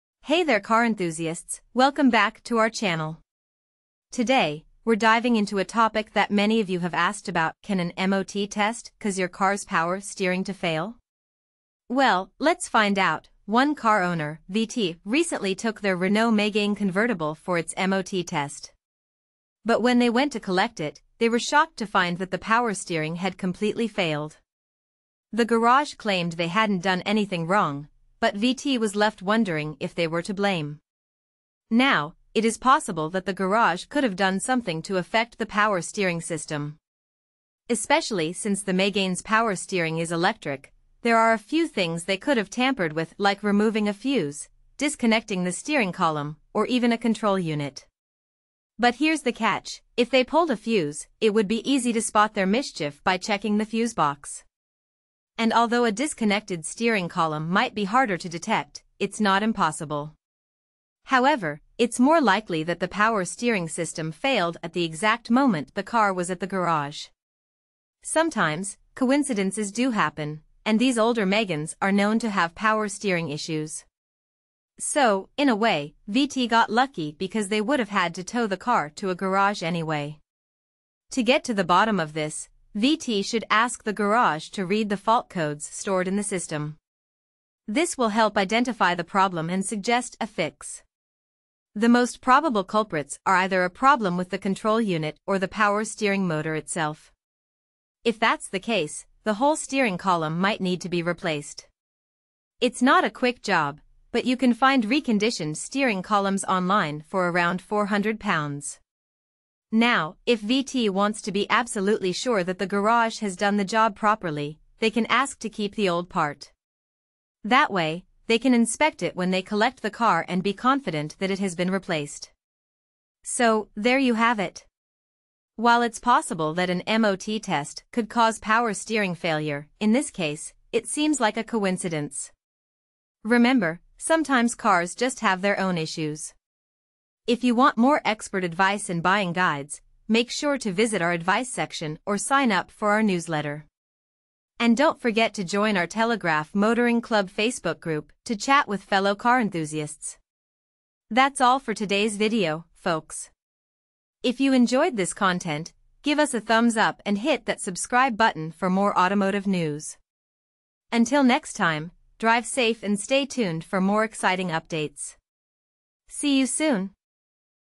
Ask the Expert: Did an MOT test cause my car’s power steering to fail?
A car owner took their Renault Megane convertible for an MOT test and upon collection, the garage informed them that the power steering had failed completely. The owner suspects that the garage did something to cause the failure. However, the expert suggests that it is more likely that the power steering system failed coincidentally at the garage. The expert explains that Meganes of this age can suffer from power steering issues, so it wouldn't be surprising. They recommend reading the fault codes stored in the system to determine the problem and suggest that it could be a problem with the control unit or the power steering motor, requiring the replacement of the whole column. The expert advises the owner to ask the garage to keep the old part so they can see it when they collect the car to ensure that it has been replaced. Some commenters share similar experiences of cars coming out of the garage with new problems, but the general consensus is that these incidents are likely coincidental or due to the age and condition of the car. In France, MOT centers are independent of garages and cannot undertake repair work, eliminating the incentive for them to create problems for repair profit.